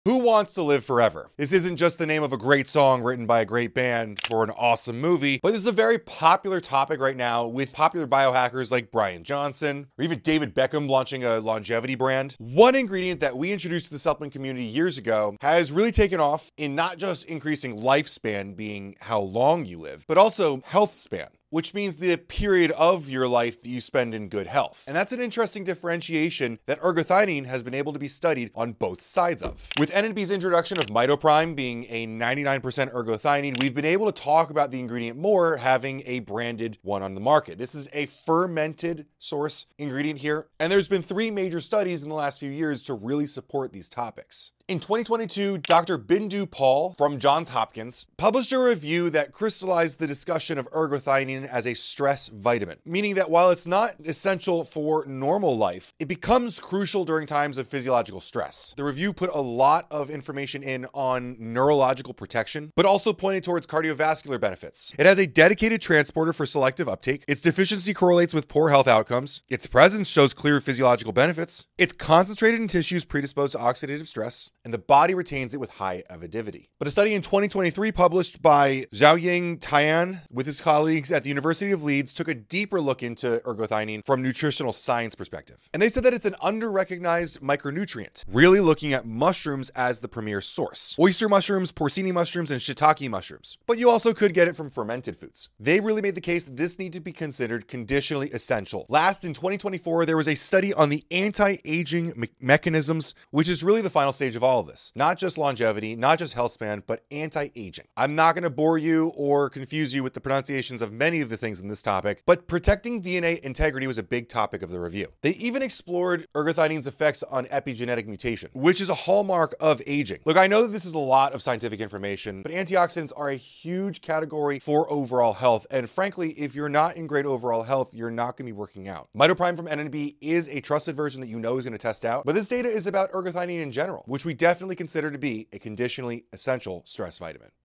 MitoPrime Ergothioneine: The Longevity Vitamin Backed By 3 Major Scientific Studies
Three groundbreaking scientific reviews now confirm Ergothioneine as a crucial "longevity vitamin" for healthy aging. This unique amino acid protects cells from oxidative damage, supports brain health, and maintains DNA integrity as we age.
MitoPrime by @nnbnutrition157  delivers pharmaceutical-grade L-Ergothioneine (99% pure) through advanced fermentation - the highest quality available on the market.
Have you added this powerful antioxidant to your supplement routine? Let us know your experience in the comments!